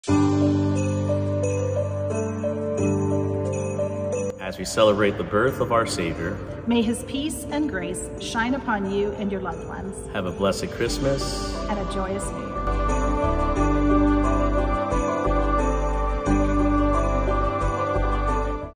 Merry Christmas from St Mary's!
Fr. Vince and Principal Gina Stull wish us a very Merry Christmas!